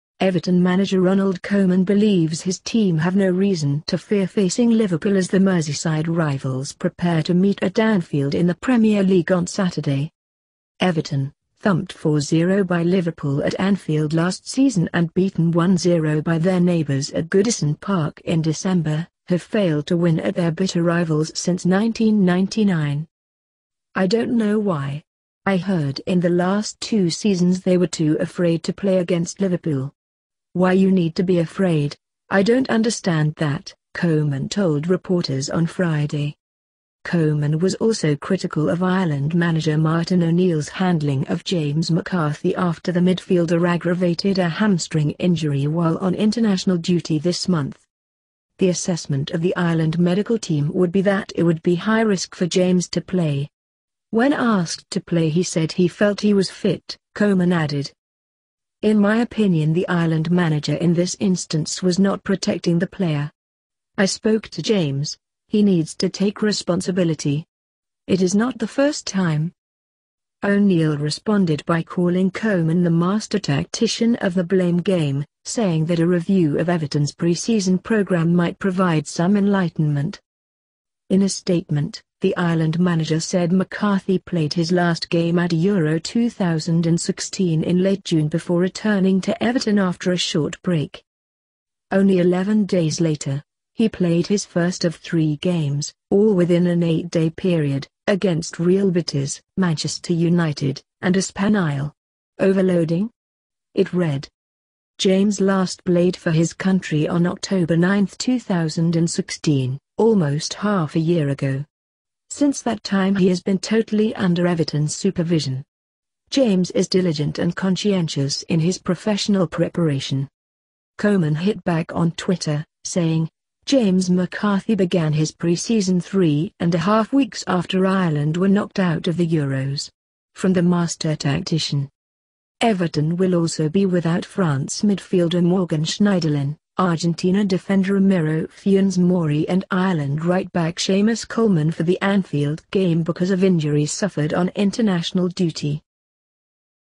Everton have no reason to fear Liverpool, says Ronald Koeman
verton, thumped 4-0 by Liverpool at Anfield last season. (Source: Reuters)
Everton manager Ronald Koeman believes his team have no reason to fear facing Liverpool as the Merseyside rivals prepare to meet at Anfield in the Premier League on Saturday.
Everton, thumped 4-0 by Liverpool at Anfield last season and beaten 1-0 by their neighbours at Goodison Park in December, have failed to win at their bitter rivals since 1999.
“I don’t know why. I heard in the last two seasons they were too afraid to play against Liverpool. Why you need to be afraid, I don’t understand that,” Koeman told reporters on Friday.
Koeman was also critical of Ireland manager Martin O’Neill’s handling of James McCarthy after the midfielder aggravated a hamstring injury while on international duty this month.
“The assessment of the Ireland medical team would be that it would be high risk for James to play. When asked to play he said he felt he was fit,” Koeman added.
“In my opinion the Ireland manager in this instance was not protecting the player. I spoke to James, he needs to take responsibility… It is not the first time.”
O’Neill responded by calling Koeman the “master tactician of the blame game”, saying that a review of Everton’s pre-season programme might “provide some enlightenment”.
In a statement, the Ireland manager said McCarthy played his last game at Euro 2016 in late June before returning to Everton after a short break.
“Only 11 days later, he played his first of three games, all within an eight-day period, against Real Betis, Manchester United and Espanyol. Overloading?” it read.
“James last played for his country on October 9, 2016 – almost half a year ago. Since that time he has been totally under Everton’s supervision. James is diligent and conscientious in his professional preparation.”
Koeman hit back on twitter, saying: “James McCarthy began his pre-season three and a half weeks after Ireland were knocked out of the Euros. From the master tactician.”
Everton will also be without France midfielder Morgan Schneiderlin, Argentina defender Ramiro Funes Mori and Ireland right back Seamus Coleman for the Anfield game because of injuries suffered on international duty.